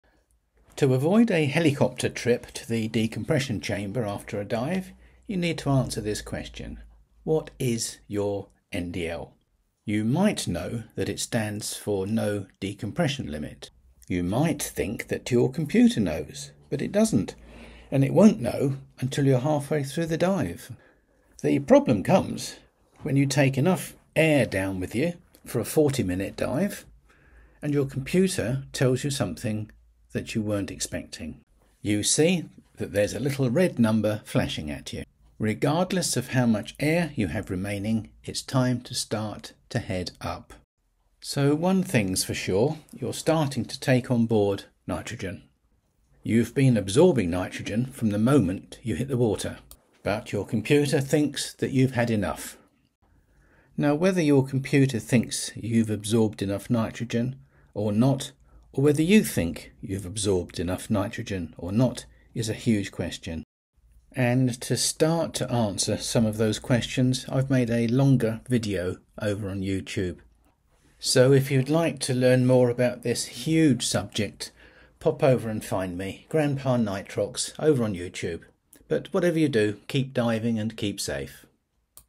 Your NDL is the key to staying safe underwater
- Understand how nitrogen absorption directly impacts your NDL.
- Discover the often-overlooked factors like hydration and thermal comfort.
- Learn why a well-rested diver can inherently enjoy longer bottom times.